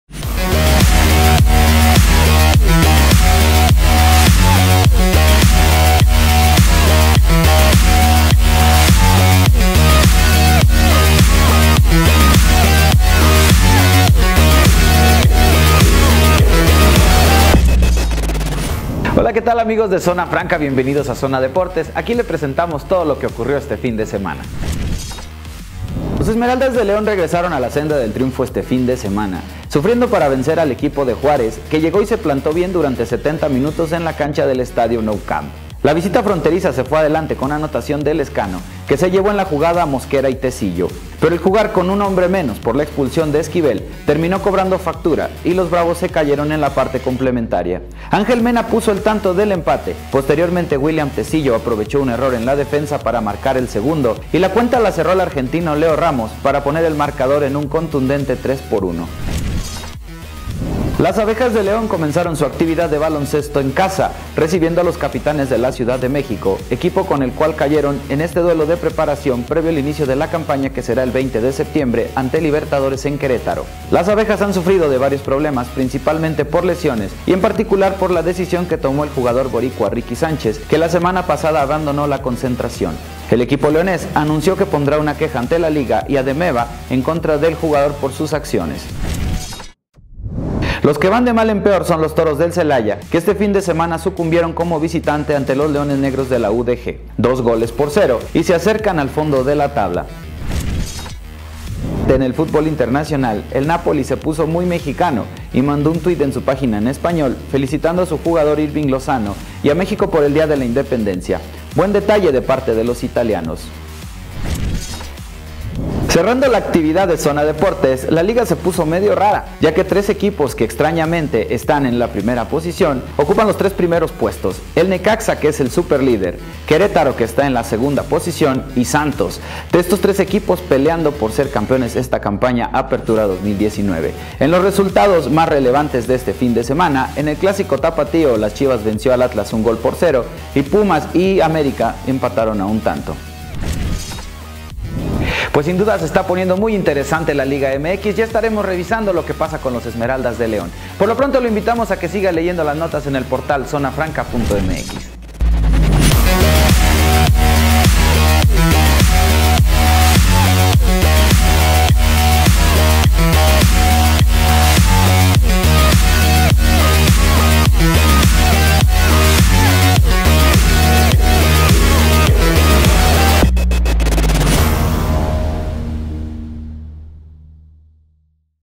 Club León vence a FC Juárez | Zona Deporte
Hoy en la ZonaDeporte⚽️ con Octavio Zúñiga
- Con sangre y lágrimas, ClubLeón derrota a FCJuárez en casa.
- Abejas pierde amistoso ante Capitanes de la CDMX.
- Celaya cae en Guadalajara ante Leones Negros y se acerca al sótano del ASCENSOMX.
- Festeja el Napoli la IndependenciaDeMéxico con imagen del ‘Chucky’ Lozano.
- Necaxa y Querétaro, líderes de la LigaMx.